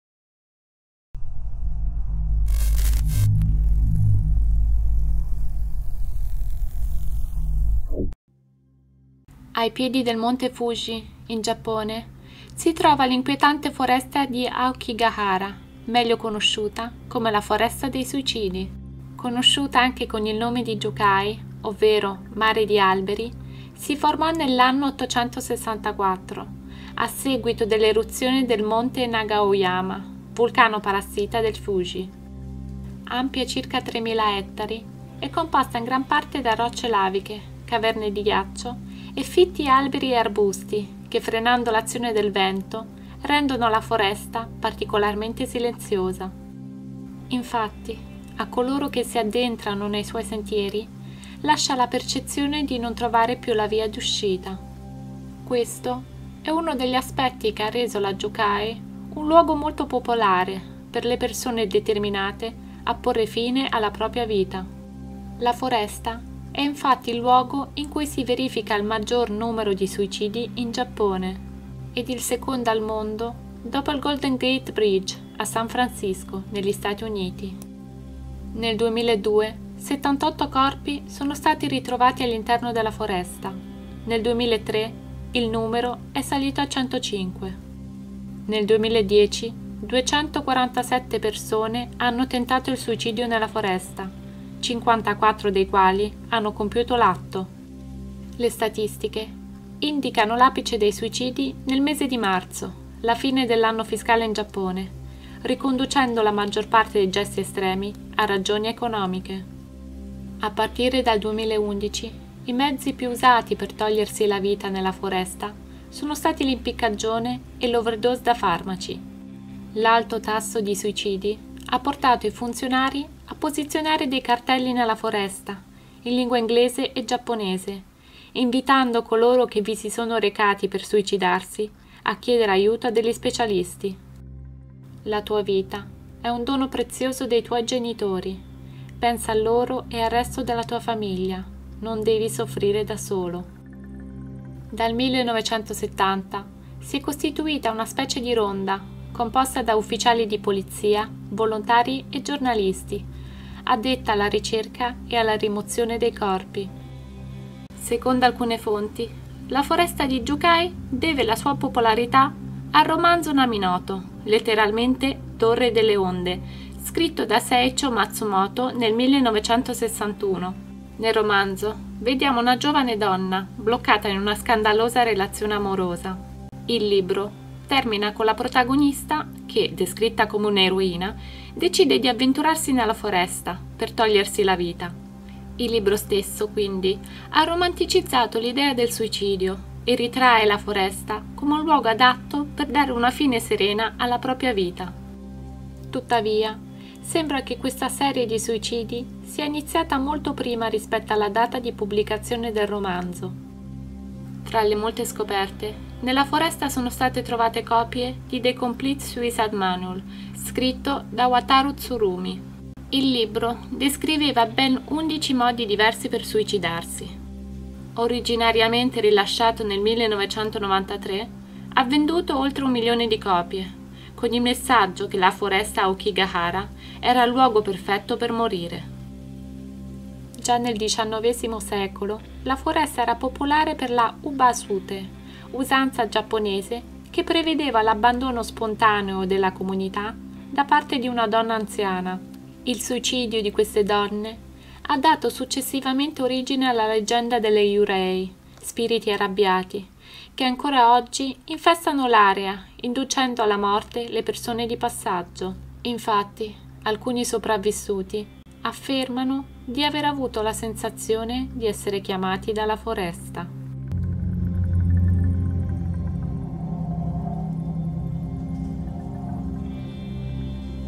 AOKIGAHARA/JUKAI - LA FORESTA DEI SUICIDI || Luoghi Inquietanti e Misteriosi del Giappone
Aokigahara, detta anche Jukai, è la foresta giapponese più famosa del mondo.

- Per chi ama i film horror, vi ricordo che esiste anche un recente film proprio riguardo a questa foresta : Jukai - La foresta dei suicidi (con Natalie Dormer come protagonista)